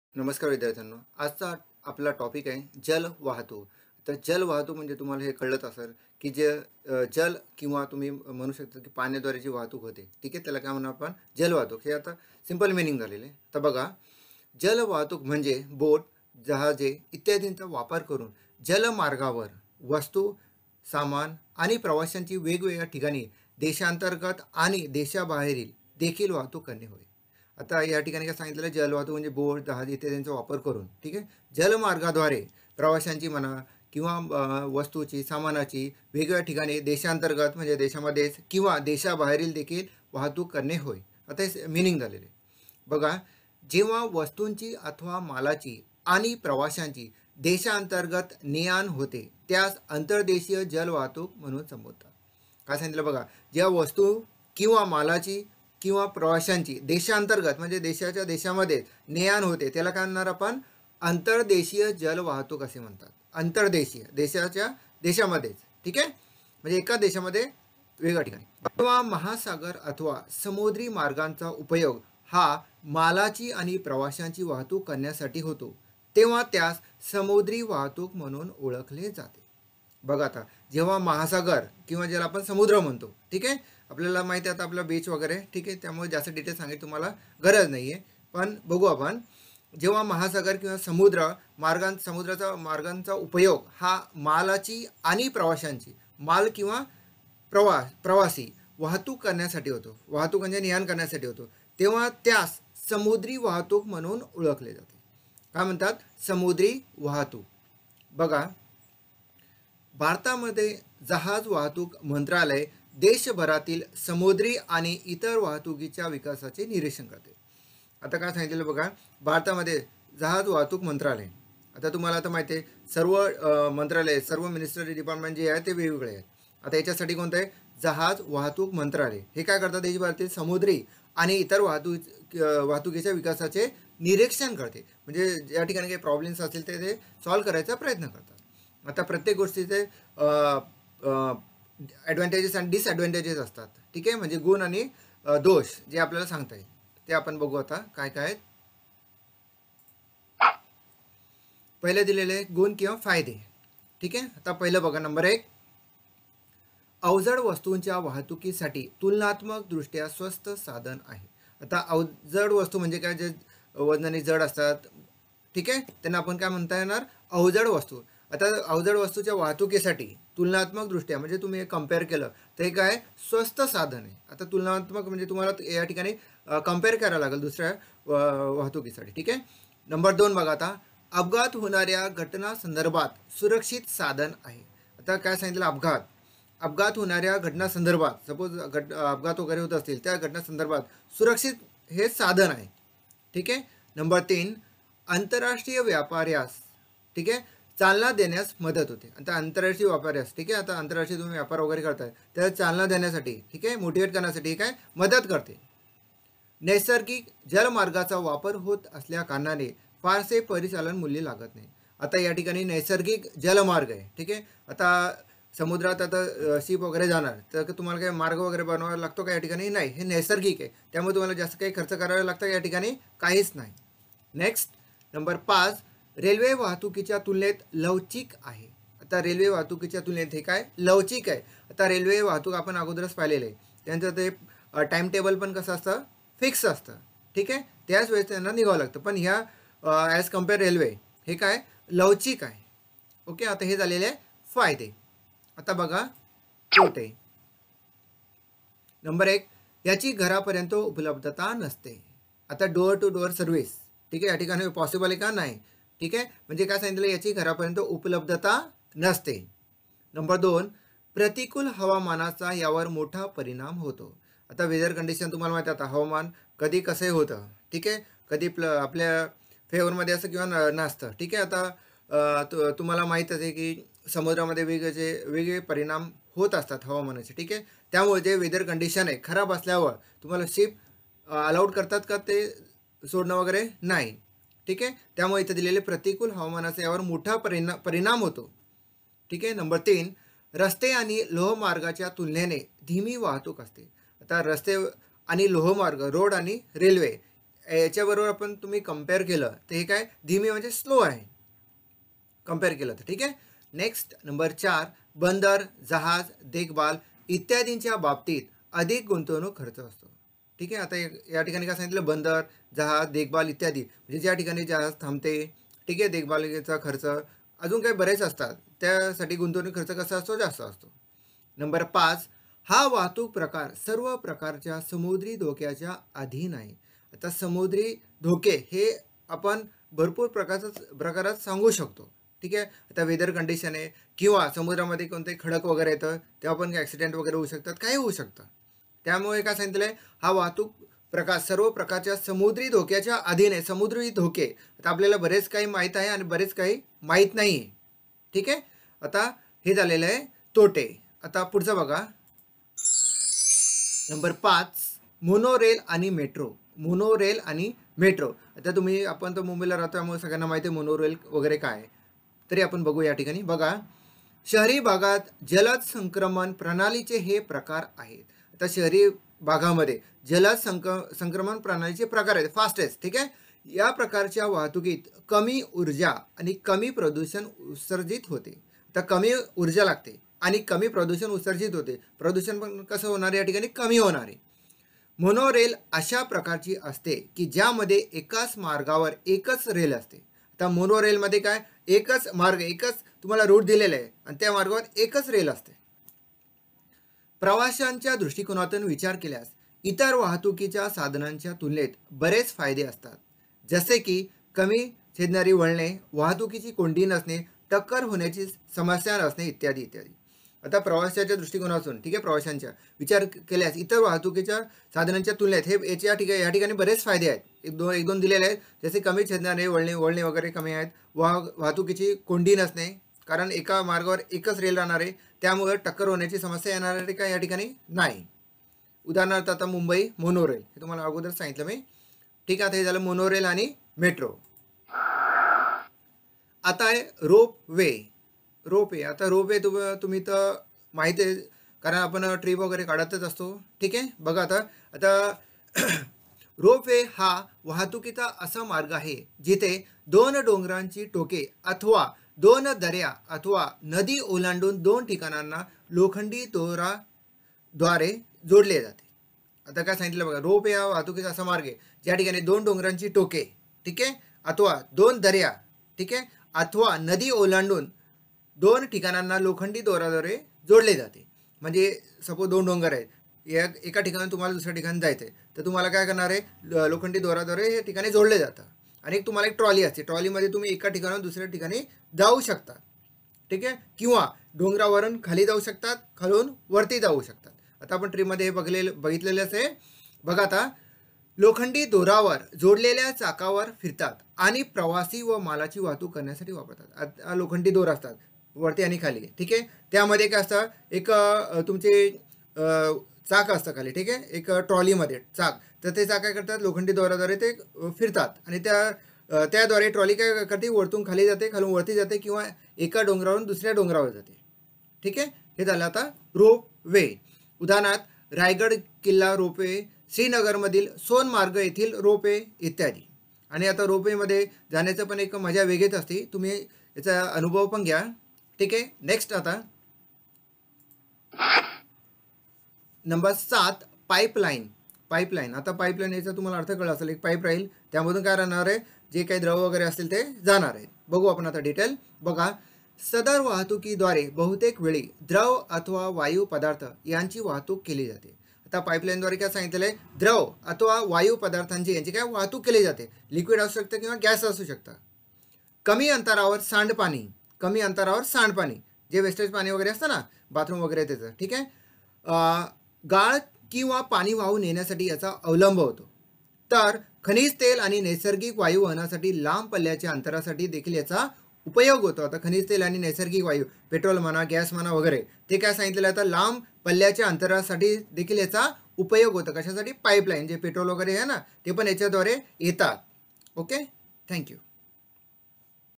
XII OCM Marathi Ch 04 (13v) व्यवसाय सेवा
जल वाहतूक, मोनोरेल आणि मेट्रो , रोपवे, पाईपलाईन वाहतूक